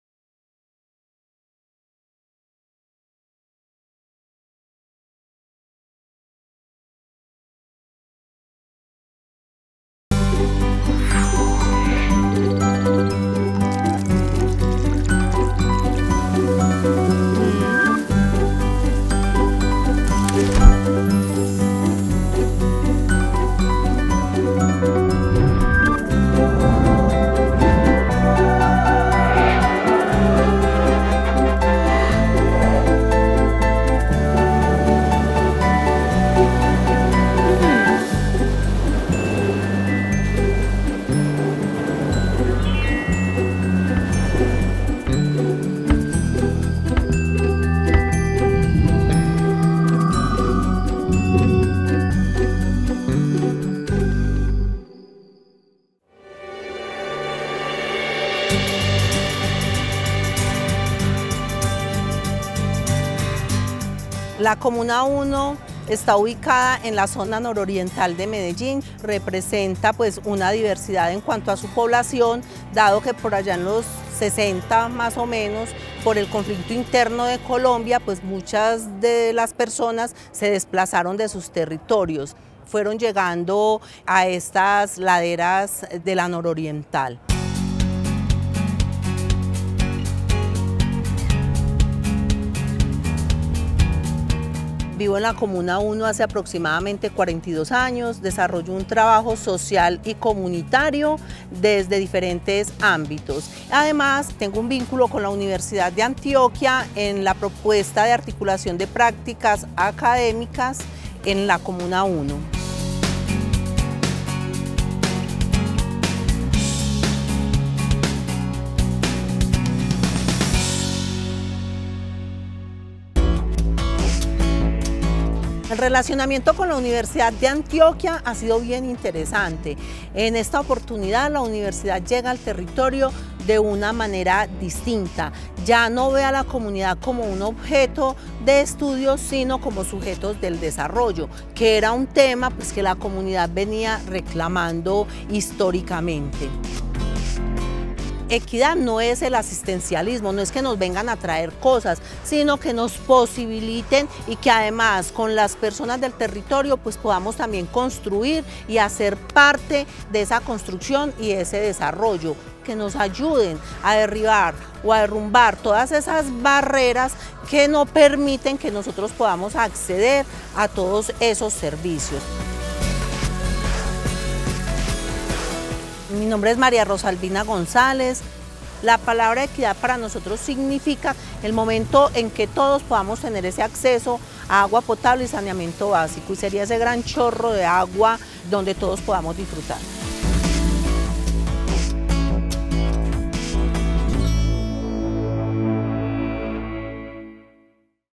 PalabrarioUdeA - Rosalbina González y la Comuna 1
Rosalbina González es lideresa en la Comuna 1 de Medellín. Con ella nos propusimos encontrar el significado de la palabra equidad, mientras trabaja como puente entre la Universidad de Antioquia y la articulación de prácticas académicas en el territorio.